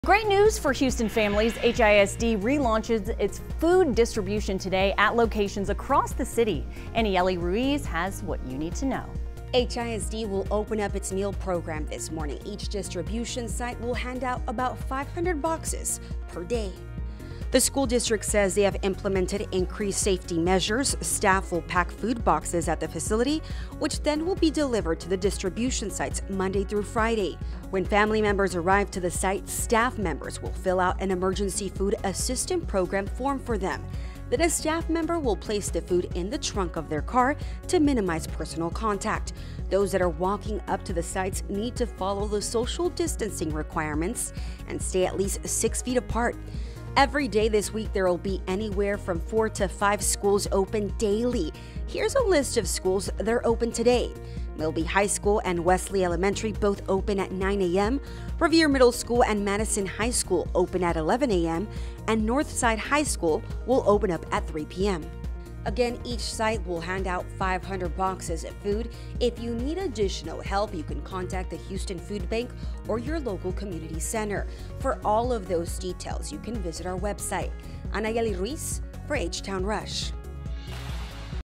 Houston ISD relaunching meal program Monday morning
Houston ISD is relaunching its meal program after the distribution sites shut down last month after a volunteer was diagnosed with coronavirus.